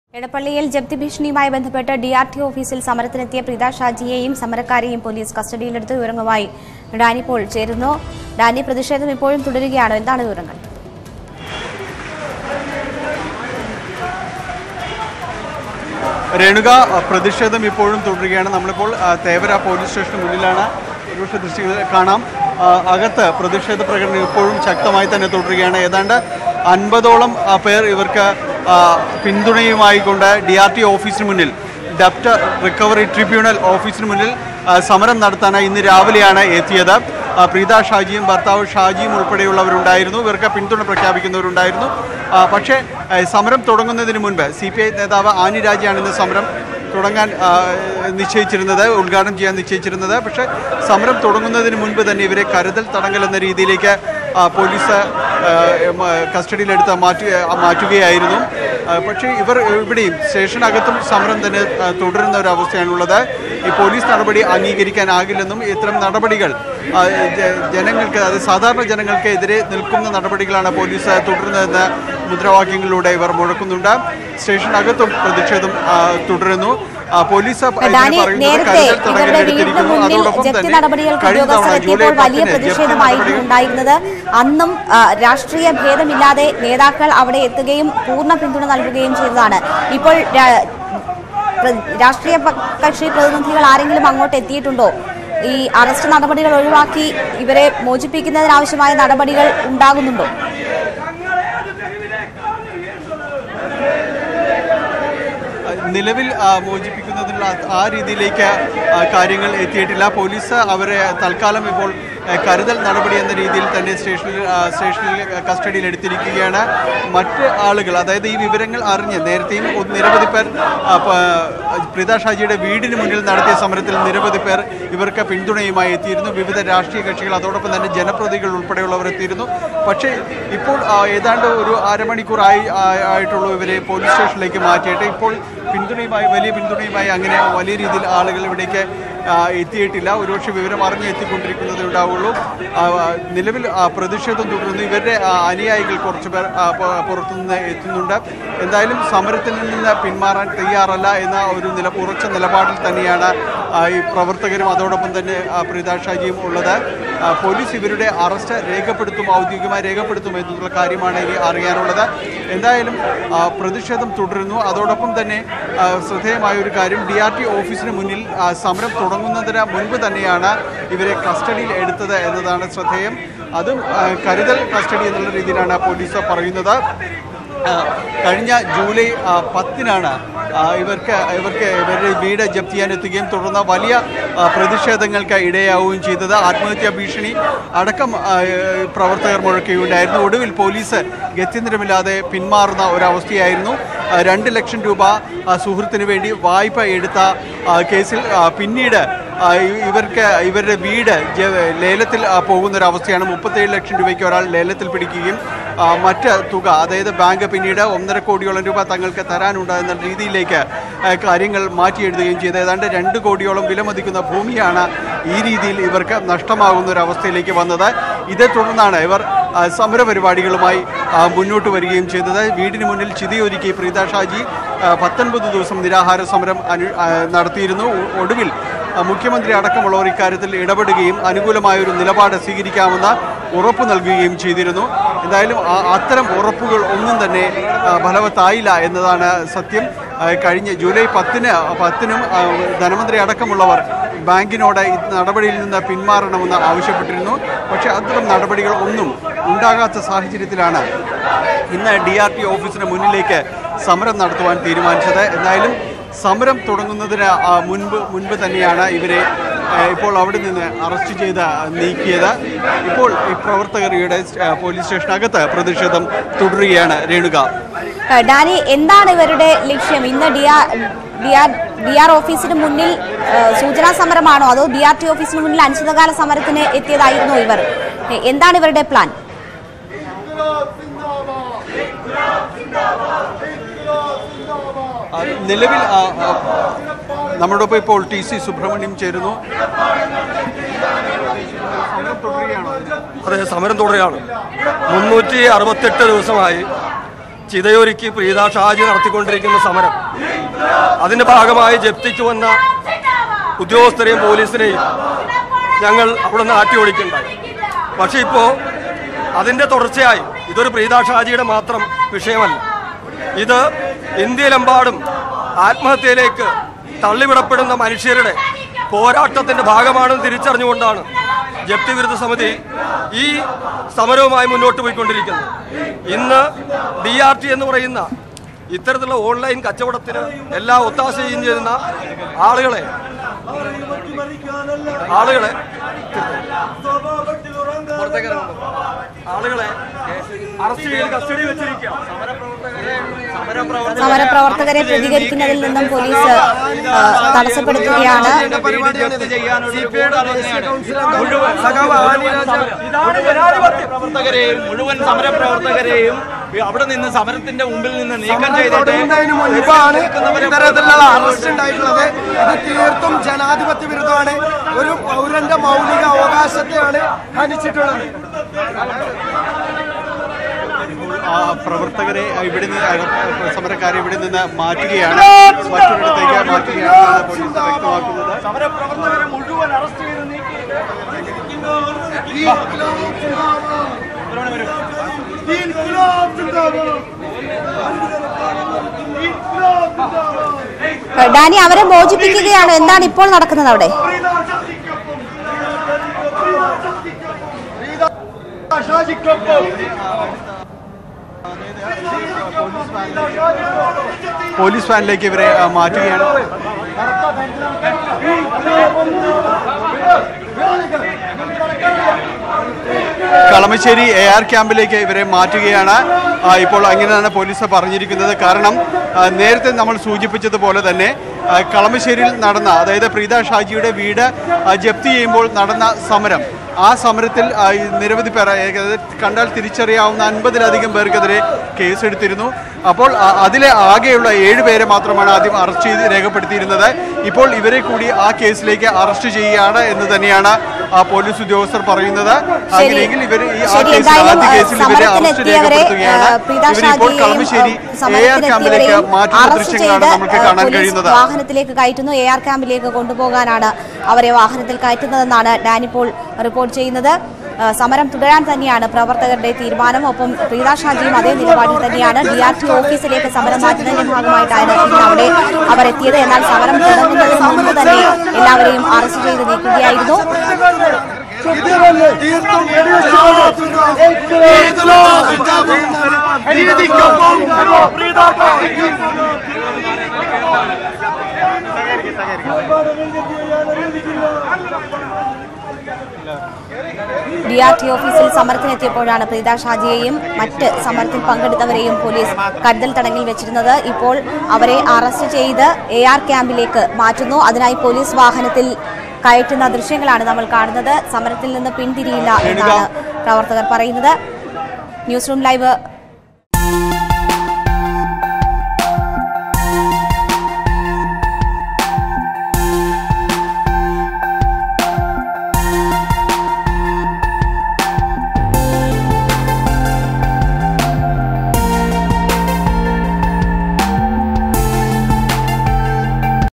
ഇടപ്പള്ളി ജപ്തി ഭീഷണി: DRT ഓഫീസിലേക്ക് സമരത്തിനെത്തിയ പ്രീത ഷാജിയും സമരക്കാരും പൊലീസ് കസ്റ്റഡിയിൽ
Police took Preetha Shaji and other protesters into coustody while they were protesting infront of the Debts Recovery Tribunal (DRT) office.